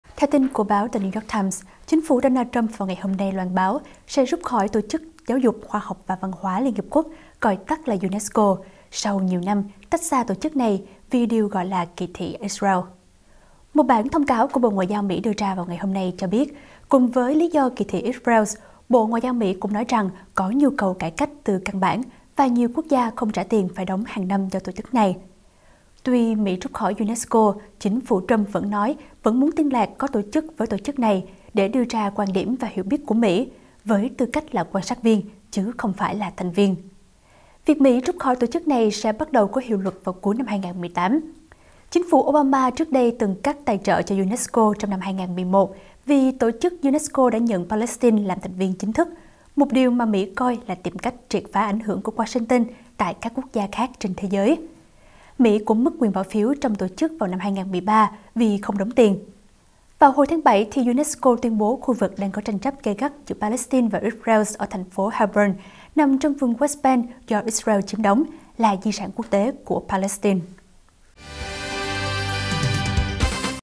Mỹ tuyên bố sẽ rút khỏi UNESCO vì tổ chức này 'kỳ thị Israel'
Chính phủ Donald Trump hôm nay loan báo sẽ rút khỏi Tổ Chức Giáo Dục, Khoa Học và Văn Hóa Liên Hiệp Quốc (gọi tắt là UNESCO), sau nhiều năm tách xa tổ chức này vì điều gọi là “kỳ thị Israel”.